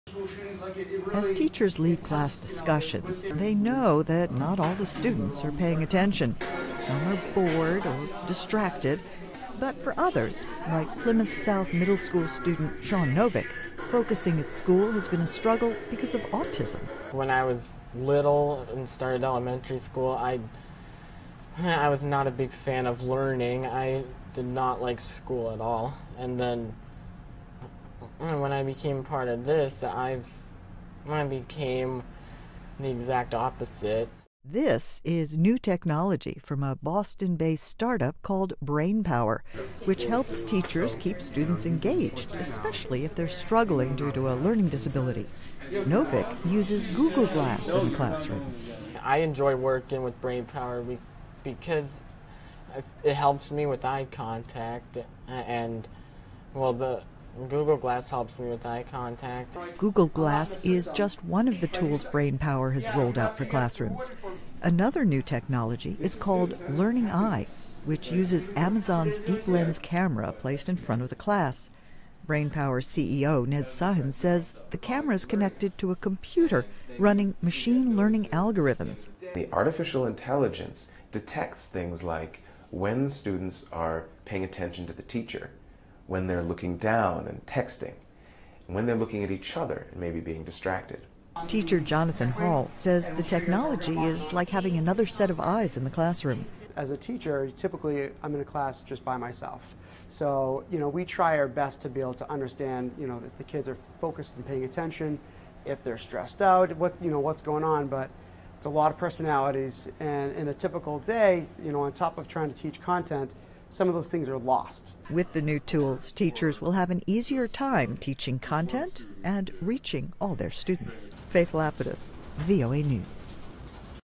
Teaching With The Newest Technology To Keep Children Interested
A school in Massachusetts is using artificial intelligence tools to help teachers keep students engaged.